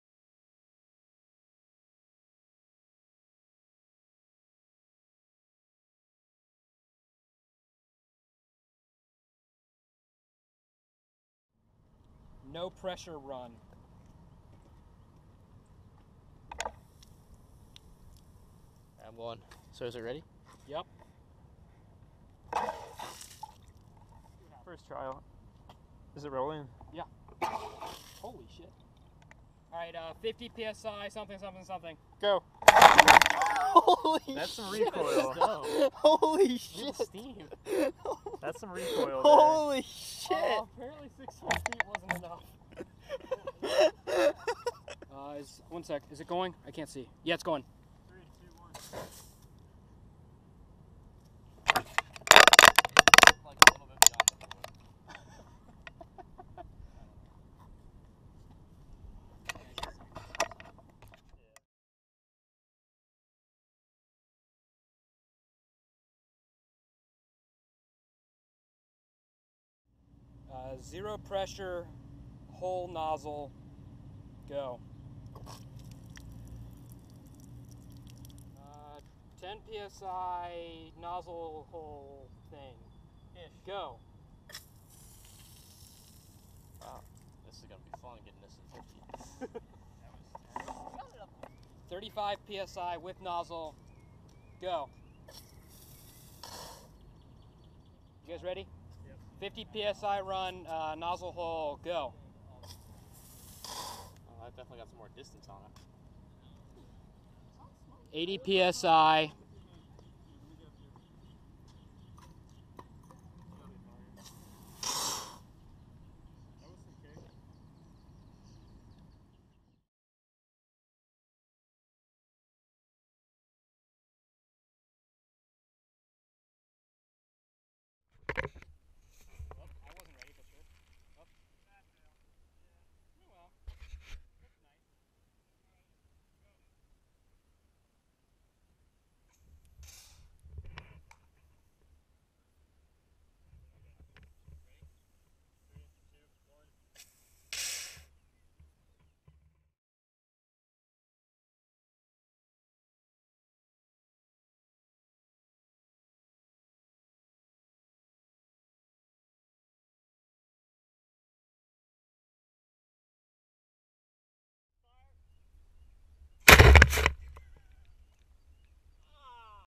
Transport 1 Project: Applications of Bernoulli's Equation for a Water Cannon
This is the testing section of our Transport 1 project.

We used build a PVC water cannon and pressurized it with a bike pump.  Then we compared the exit velocities with the theoretical values calculated from Bernoulli's Equation.

Filmed on a Go Pro Hero 3 silver